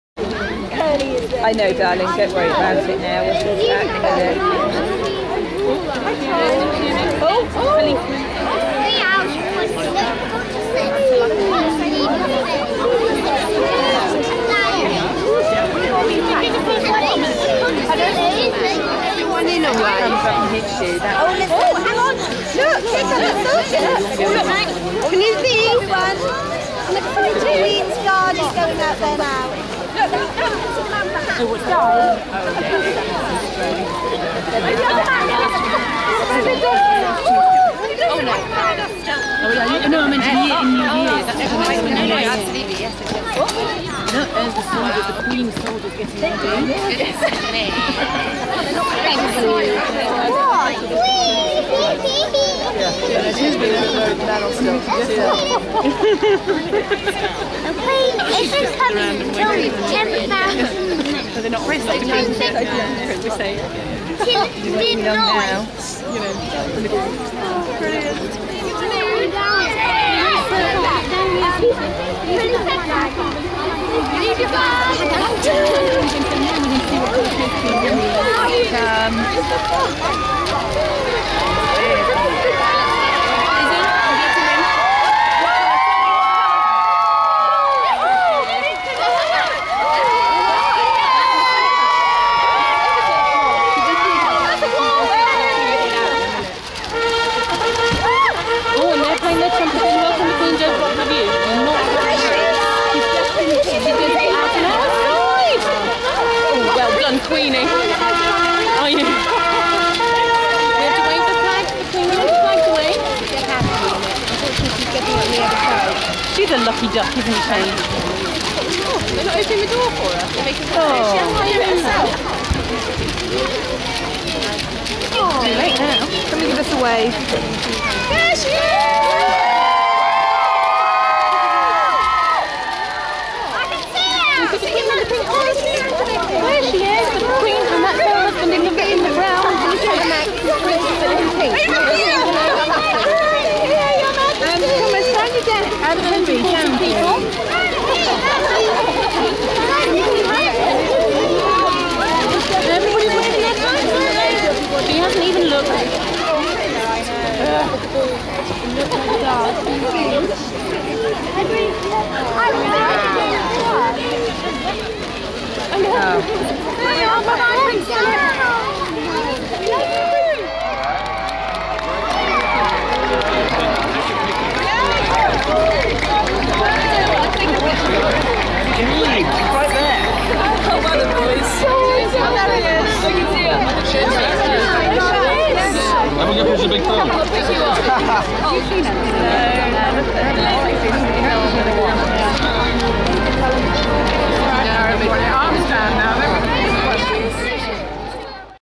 Queen in Salisbury: Diamond Jubilee 2012
Camcorder footage of Queen Elizabeth ll Visiting the Wiltshire Diamond Jubilee Celebration Event held in Salisbury Cathedral  Close on May Day 2012.  The event started amid heavy rainfall which subsided as the event progressed.  The moment Her Majesty arrived in the Cathedral Close the Sun burst through the clouds for the first time that morning and graced the event for the rest of it's duration. The Queen had lunch at the Rifles museum before visiting a student "Magna Carta" project inside Salisbury Cathedral.